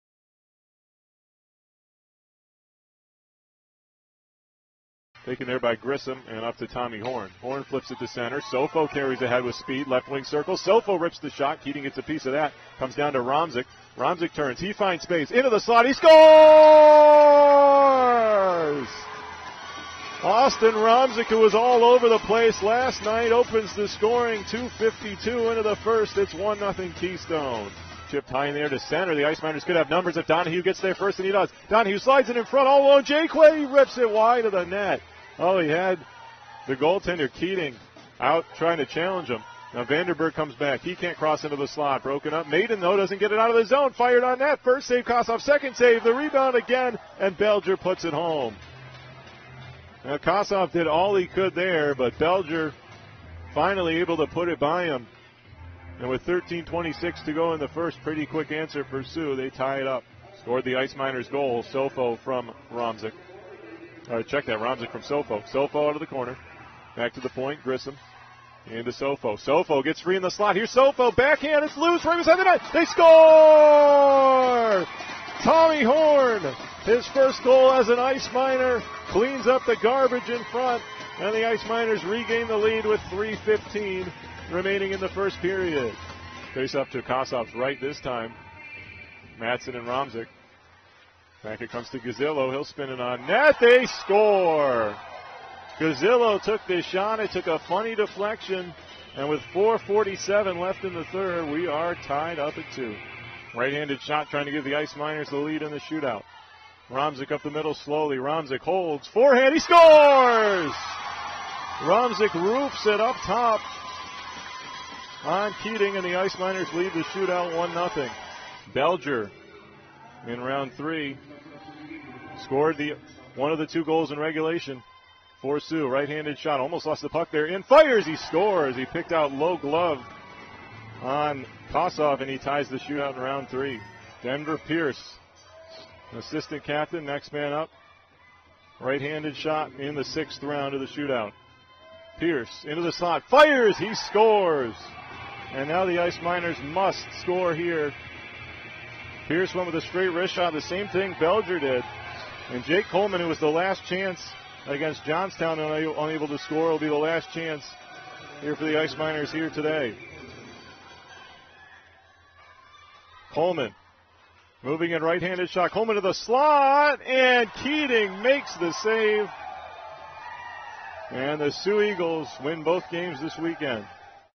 Keystone Ice Miners vs. Soo Eagles - 11/16/14 - highlights
The Ice Miners can't hold a third period lead and drop the shootout, 3-2 final score.